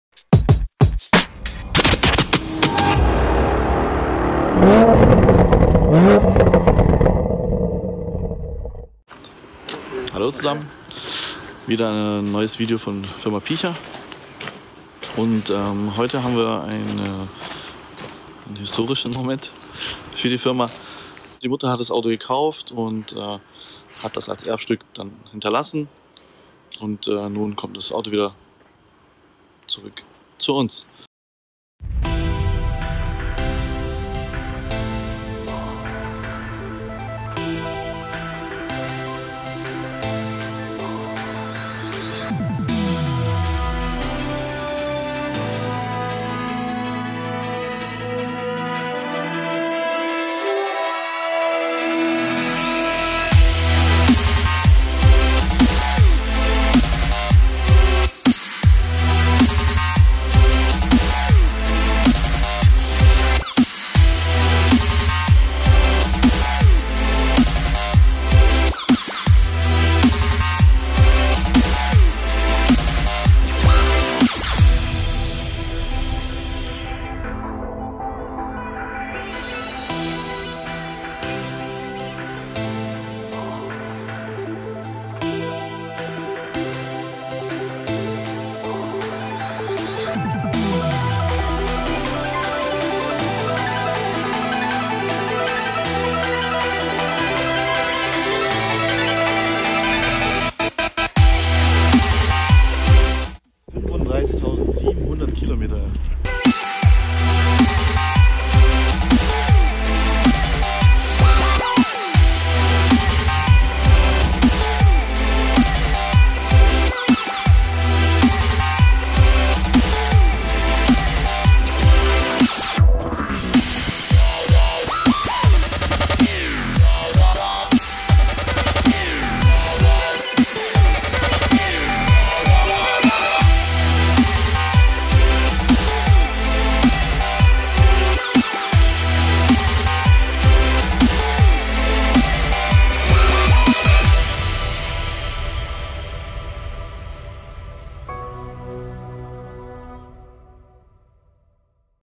PIECHA-Design-Scheunenfund: OPEL Calibra Cabrio
Nach genau 30 Jahren kommt der allererste jemals von PIECHA umgebaute OPEL Calibra CABRIO, mit der Seriennummer 001, vom Erstbesitzer zurück. Das Fahrzeug befindet sich im Originalzustand der Auslieferung vom Jahr 1990.

- Sensationell - Zustand 1A